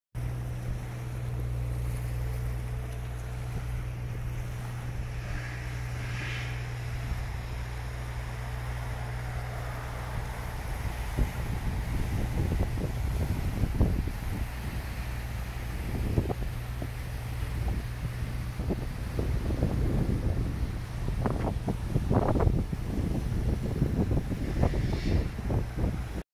2019.06.23.羽田空港の多摩川側
東京国際空港第2ゾーン計画地区。多摩川対岸への橋、ホテル等の入るビルの建設中。